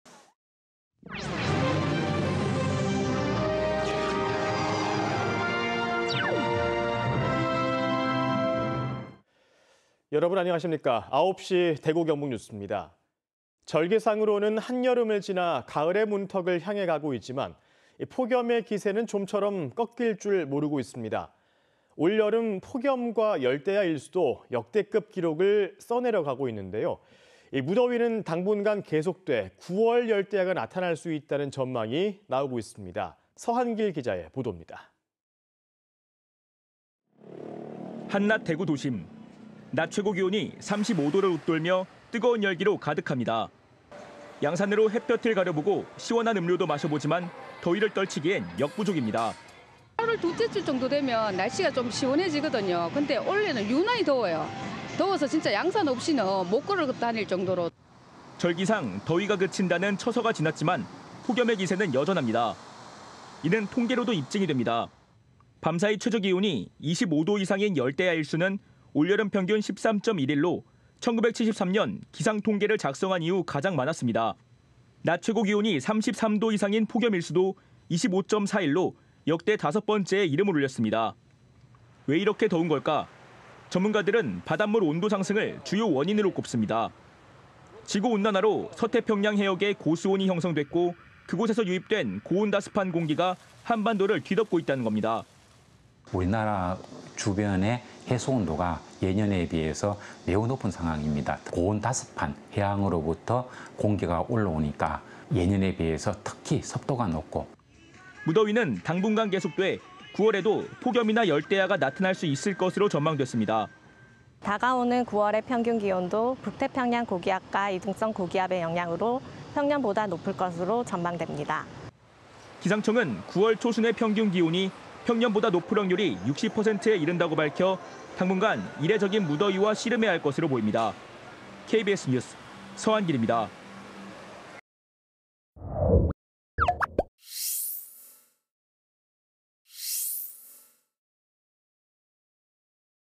‘처서’ 지났지만 여전한 폭염…9월 열대야도 가능 / KBS 2024.08.23.
[앵커]

 절기상으로는 한여름을 지나 가을의 문턱을 향해가고 있지만, 폭염의 기세는 좀처럼  꺾일 줄 모르고 있습니다.

 올 여름 폭염과 열대야 일수도  역대급 기록을 써내려가고 있는데요.

무더위는 당분간 계속돼 9월 열대야가 나타날 수 있다는 전망이 나오고 있습니다.

 서한길 기자입니다.

 [리포트]

 한낮 대구 도심.

 낮 최고기온이 35도를 웃돌며 뜨거운 열기로 가득합니다.

 양산으로 햇볕을 가려보고, 시원한 음료도 마셔보지만 더위를 떨치기엔 역부족입니다.

 [장수아/대구시 대명동 : "8월 둘째 주 정도 되면 날씨가 좀 시원해지거든요. 근데 올해는 유난히 더워요. 더워서 진짜 양산 없이는 못 걸어다닐 정도로..."]

 절기상 더위가 그친다는 처서가 지났지만 폭염의 기세는 여전합니다.

 이는 통계로도 입증이 됩니다.

 밤사이 최저기온이  25도 이상인 열대야 일수는  올 여름 평균 13.1일로, 1973년 기상 통계를 작성한 이후  가장 많았습니다.

 낮 최고기온이 33도 이상인 폭염 일수도 25.4일로 역대 5번째에 이름을 올렸습니다.

 왜 이렇게 더운 걸까?

 전문가들은 바닷물 온도 상승을 주요 원인으로 꼽습니다.

 지구 온난화로  서태평양 해역에 고수온이 형성됐고, 그곳에서 유입된 고온다습한 공기가 한반도를 뒤덮고 있다는 겁니다.

 [김해동/계명대 환경공학과 교수 : "우리나라 주변의 해수 온도가 예년에 비해서 매우 높은 상황입니다. 고온다습한 해양으로부터 공기가 올라오니까 예년에 비해서 특히 습도가 높고..."]

 무더위는 당분간 계속돼 9월에도 폭염이나 열대야가  나타날 수 있을 것으로 전망됐습니다.

 [이지혜/대구지방기상청 주무관 : "다가오는 9월의 평균기온도 북태평양고기압과 이동성 고기압의 영향으로 평년보다 높을 것으로 전망됩니다."]

 기상청은 9월 초순의 평균 기온이 평년보다 높을 확률이  60%에 이른다고 밝혀, 당분간 이례적인 무더위와  씨름해야 할 것으로 보입니다.

 KBS 뉴스 서한길입니다.

 촬영기자:백재민